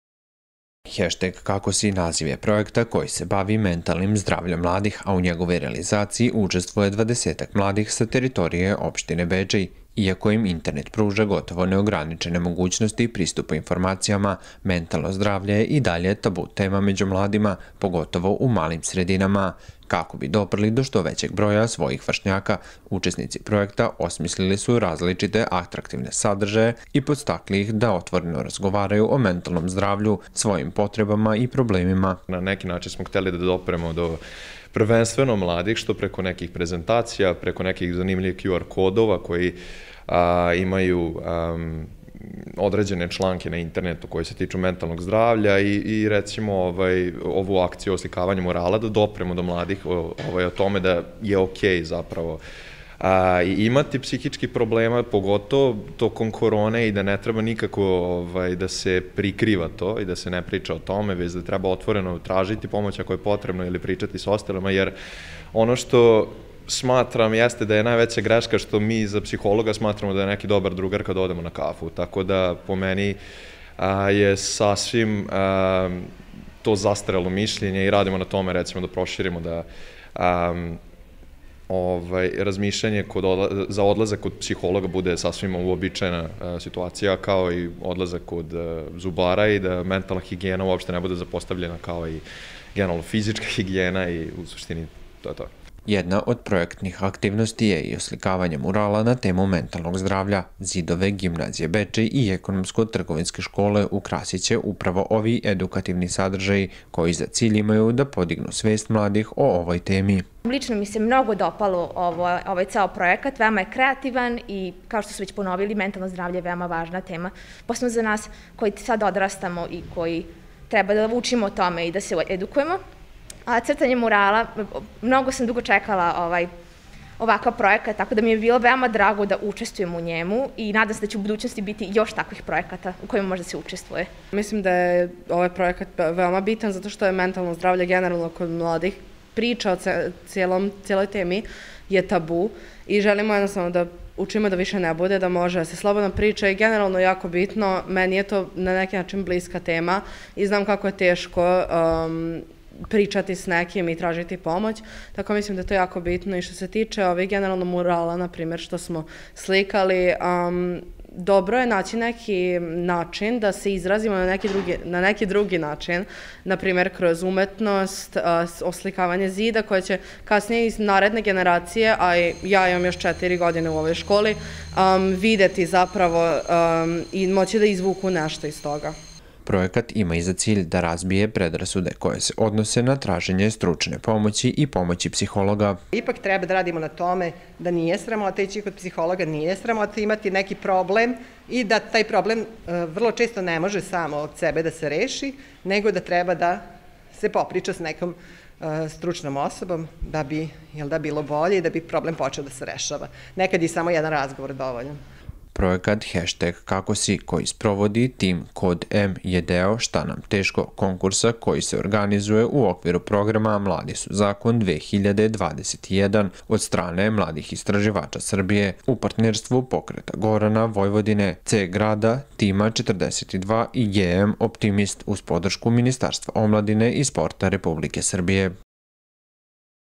TV BEČEJ: Mentalno zdravlje i dalje tabu tema među mladima
TV BEČEJ: “Hešteg KAKO SI?” naziv je projekta koji se bavi mentalnim zdravljem mladih, a u njegovoj realizaciji učestvuje dvadesetak mladih sa teritorije opštine Bečej.a